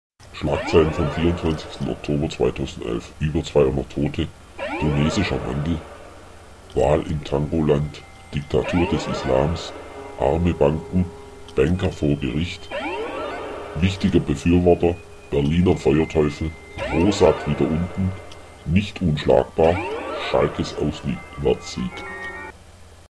Nachrichten vom 24.10.2011
Genauere Informationen zu den 24 Oktober 2011 Schoenes Wetter Com Schlagzeilen auf:
- Ueber 200 tot ?, Tunesischer Wandel, Wahl im Tangoland, Diktatur des Islams, Arme Banken, Banker vor Gericht, Wichtiger Befuerworter, Berliner Feuerteufel, Rosat wieder unten, Nicht unschlagbar, Schalkes Auswaertssieg